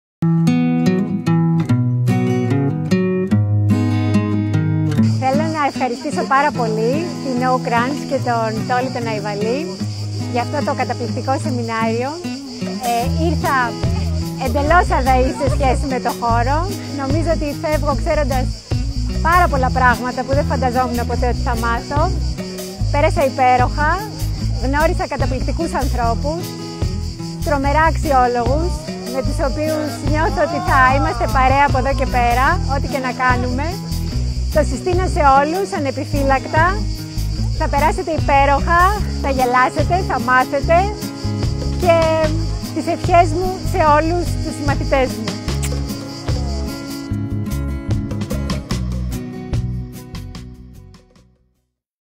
Mania Kanaki -graduate of Diploma in Digital & Social Media
Mania Kanaki - Positive Psychology Coach,graduate of knowcrunch.com, talks about